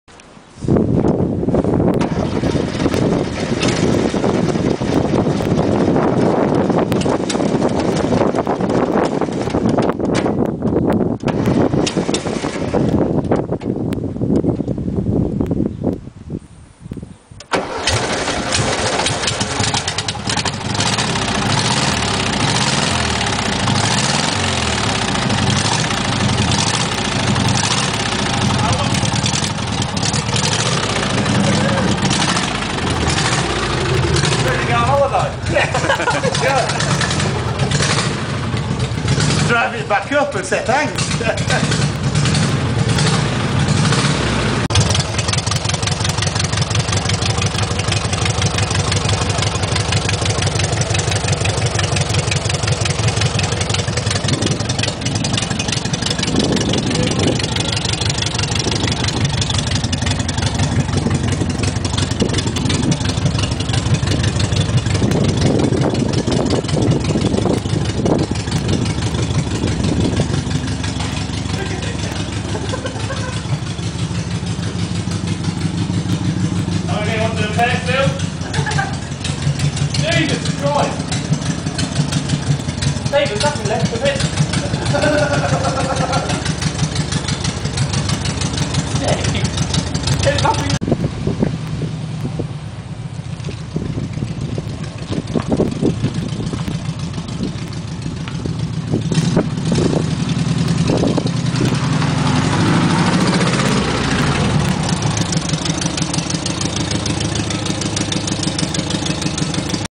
Bay window wreck (from Camper&Bus Jan 11 issue)
When someone tells you there's an old Camper sat waiting to be saved it would be rude not to accept the challenge of bringing it back from the dead.
This video shows the Bay window in question - it had been sat a long time and is very very rotten. But it still started with no tinkering required!